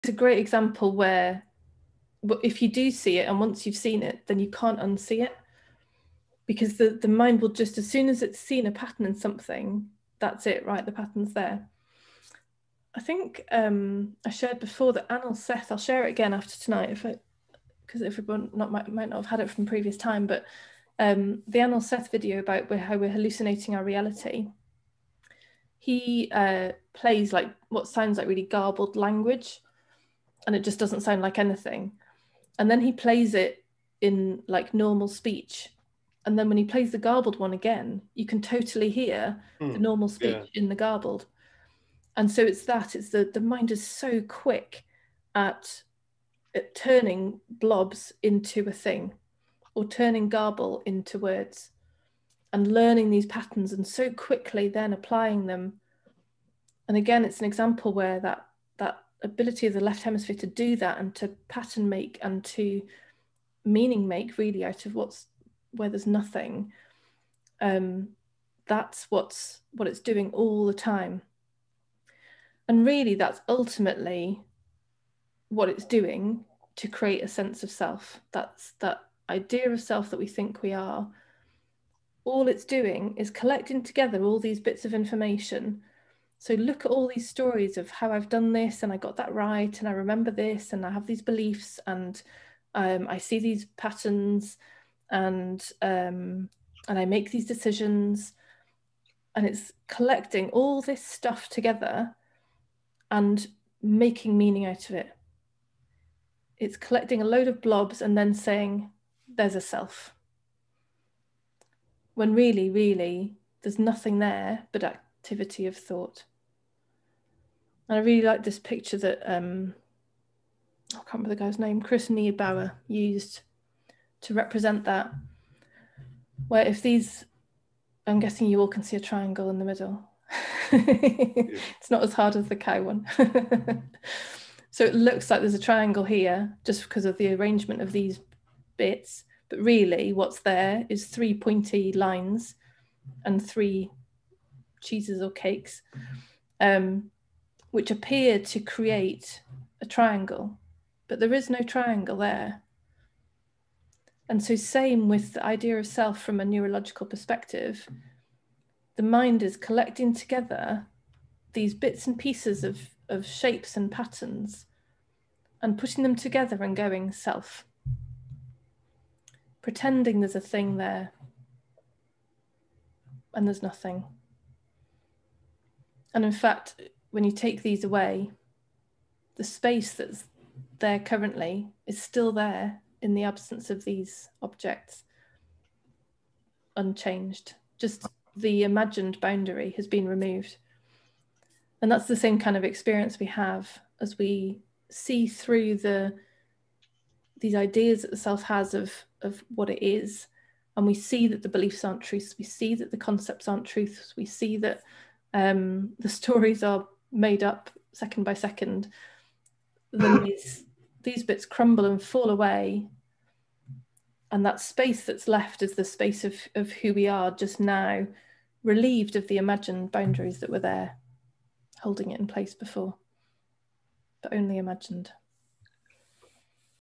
The idea of self is the result of a pattern-making mind
Who you think you are is a mirage - it feels real, there really seems to be a character with your name and with these traits and preferences, but in fact it's a construct of a mind that loves to find a pattern where there is none. A mind which loves to create a concept or an abstract representation of what's actually going on.

All of this disconnects us from the moment as we're lost within a confused mind taking ideas as real objects.